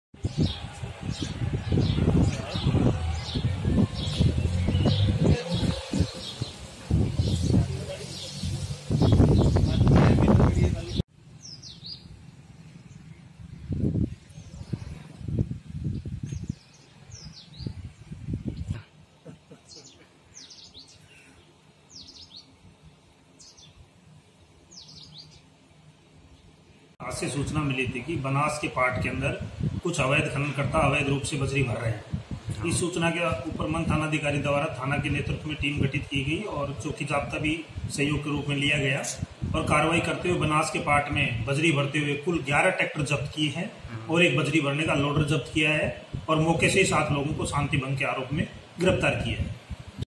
SAWAI MADHOPUR में अवैध बजरी खनन पर पुलिस की कार्रवाई
सवाई माधोपुर में अवैध बजरी खनन पर पुलिस का डंडा चला. चौथ का बरवाड़ा थाना और शिवाड़ पुलिस की संयुक्त कार्रवाई के दौरान पुलिस ने 11 ट्रेक्टर टोली और एक लोडर को जब्त किया. इस कार्रवाई के बाद बजरी माफिया में हड़कंप मच गया. दरअसल पुलिस को अवैध बजरी खनन की सूचना मिली जिसके बाद पुलिस ने टीम गठित कर कार्रवाई को अंजाम दिया...इसी दौरान पुलिस ने सात चालकों को भी शांतिभंग के आरोप में गिरफ्तार किया है.